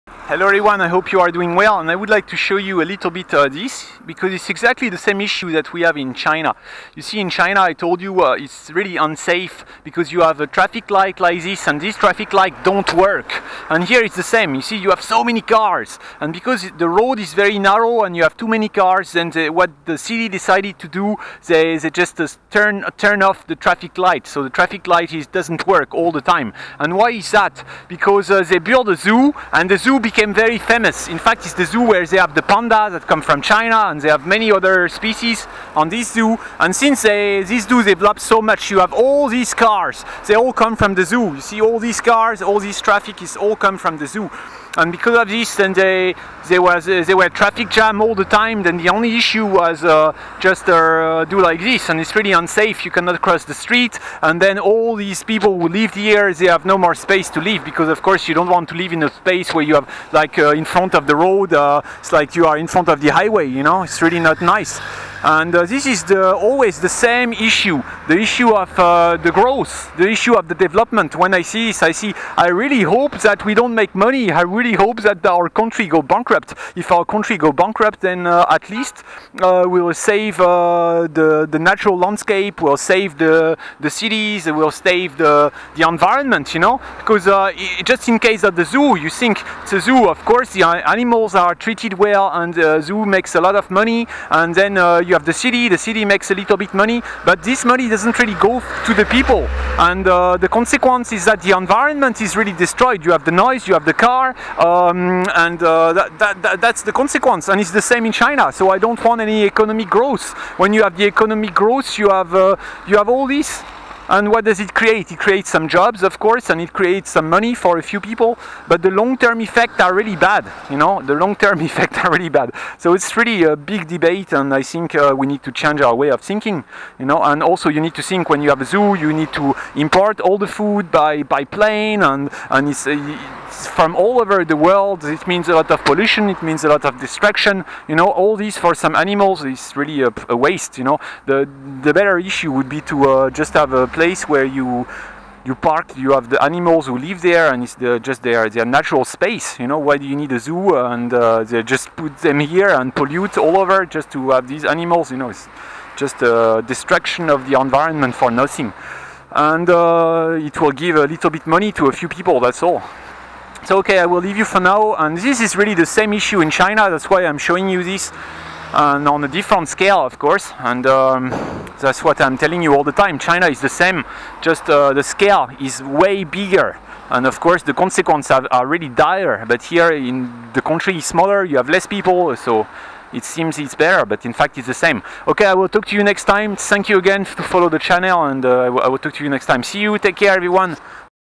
Why I hate the growth model: too much traffic... turn off the traffic light!
In China quite often I see traffic lights turned off in order to ease the traffic. BECAUSE there are too many cars, too many people and they follow a growth model with all that it implies: pollution, global destruction...